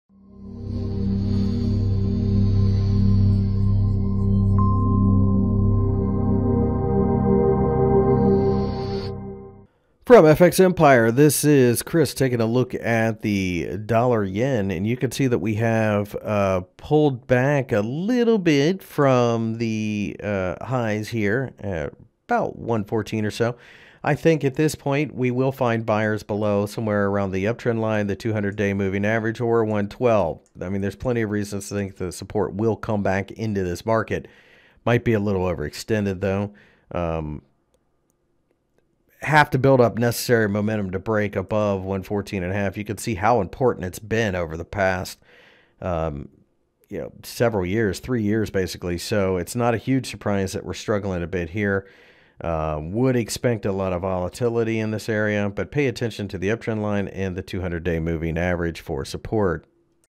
USD/JPY Technical Analysis for November 30, 2018 by FXEmpire.com
The US dollar fell a bit during the trading session on Thursday, reaching down towards the ¥113 level before finding... For the full article go - USD/JPY Forecast November 30, 2018, Technical Analysis.Find more information about Forex and Commodities News Technical Analysis Fundamental Analysis and Brokers on FX Empire